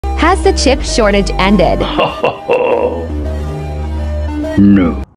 When will the chip shortage end?
The question we all keep asking at the moment. when will the chip shortage end? With the delays likely to last well into the last quarter of this year, have you been impacted?

Fancy a FREE Leasing Guide? 👀